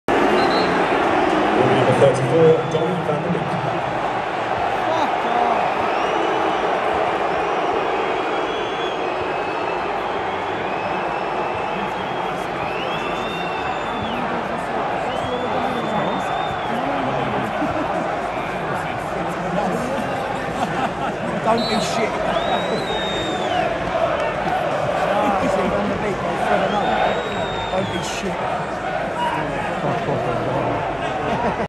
Bruno Fernandes being substituted for Donny van de Beek
Manchester United vs Chelsea FC